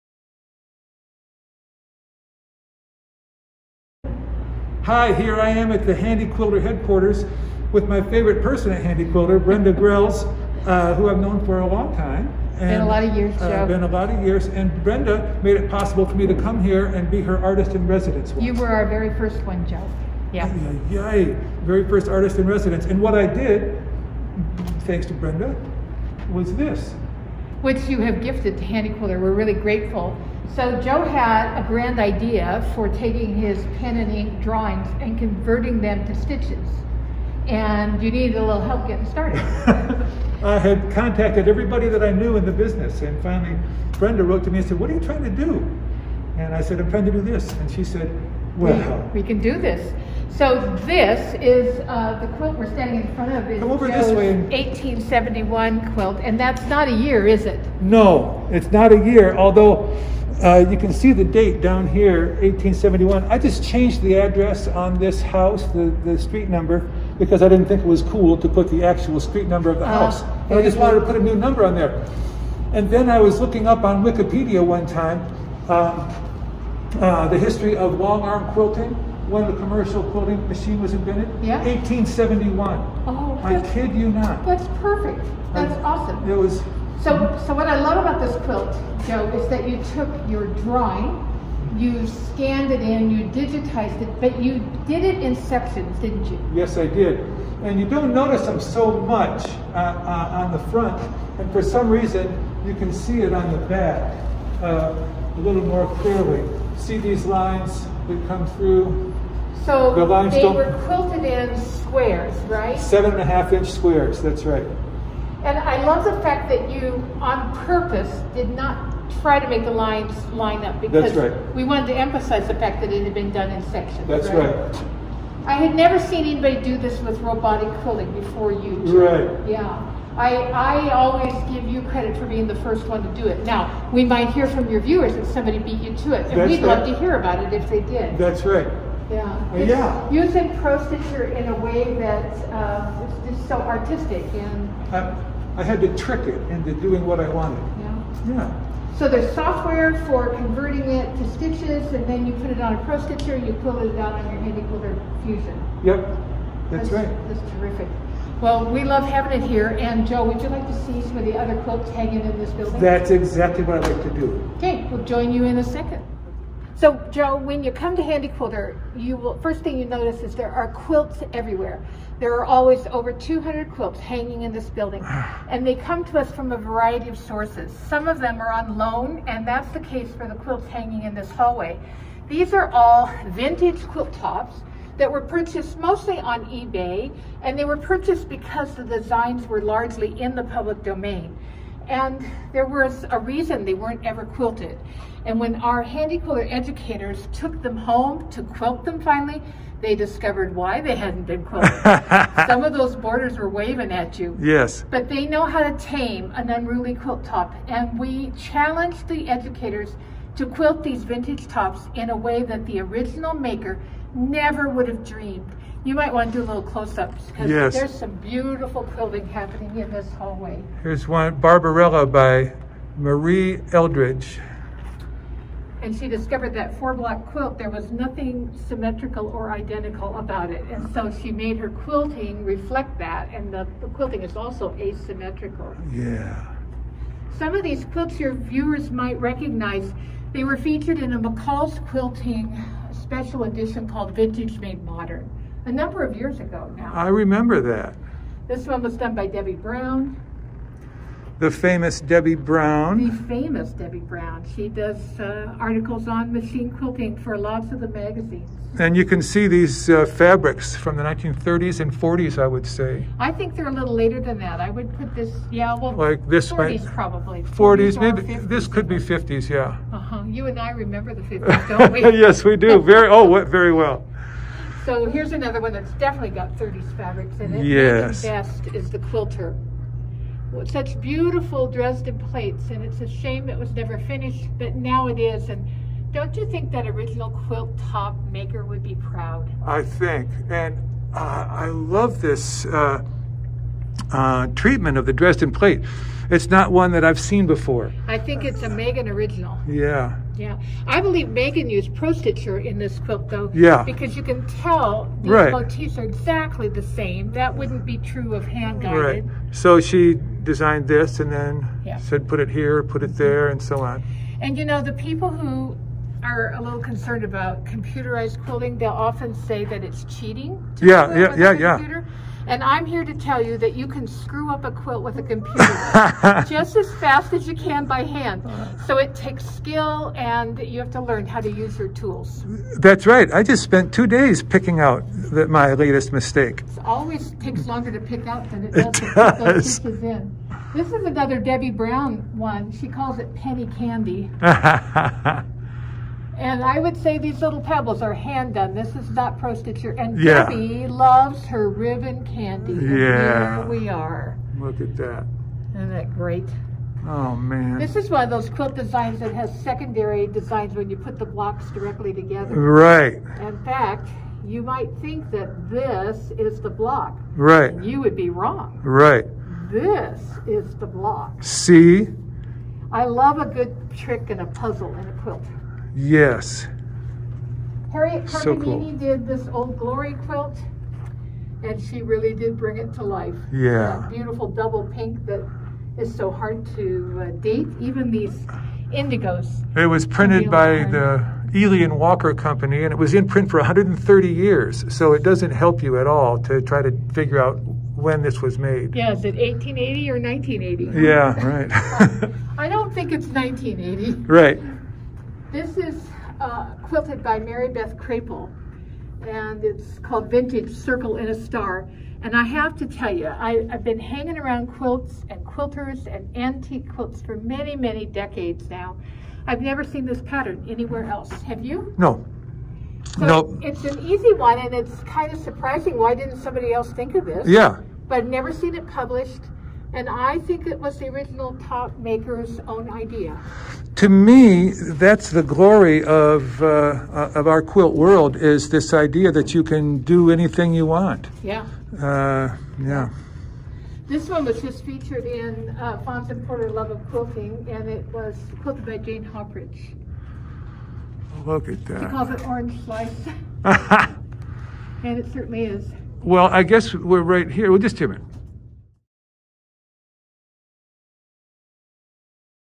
The Quilt Report 64
Part one of my visit to see a couple of my own quilts and some of the more than 200 quilts on display at the Handi Quilter Headquarters in North Salt Lake.